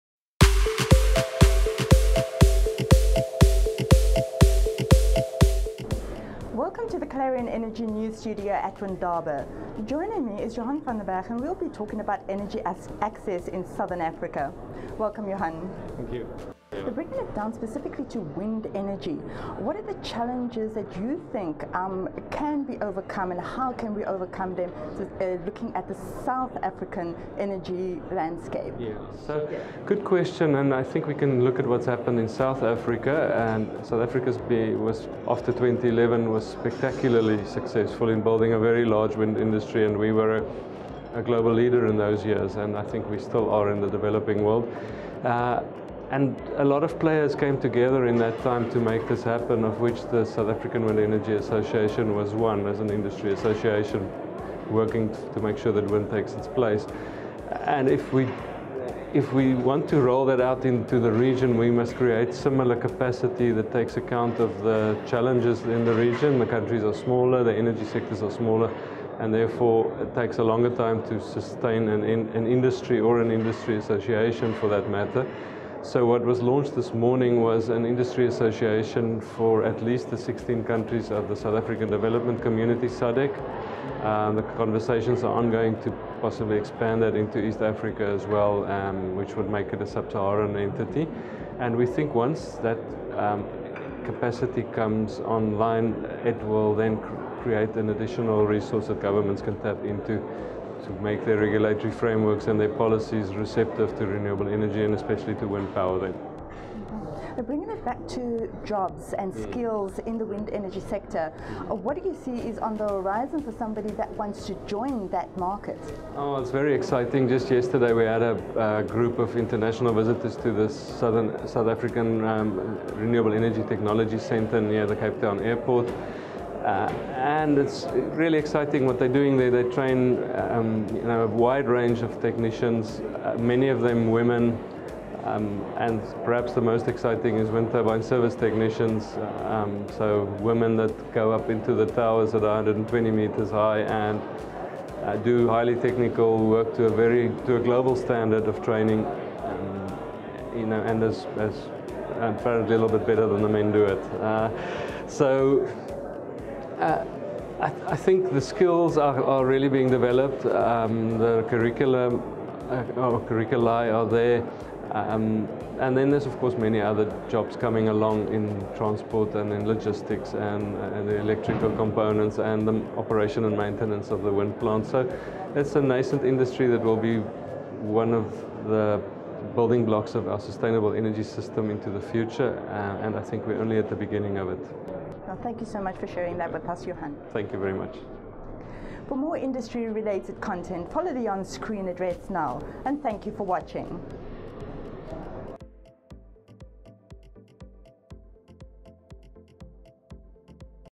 Windaba 2019: Creating an ecosystem for energy success in Southern Africa
The energy markets in Southern African countries are smaller and therefore it takes longer to sustain an industry, however, governments can lead by making regulatory frameworks and policies receptive to renewable energy says Johan van den Berg, head of the AEEP Secretariat.

Video produced by ESI Africa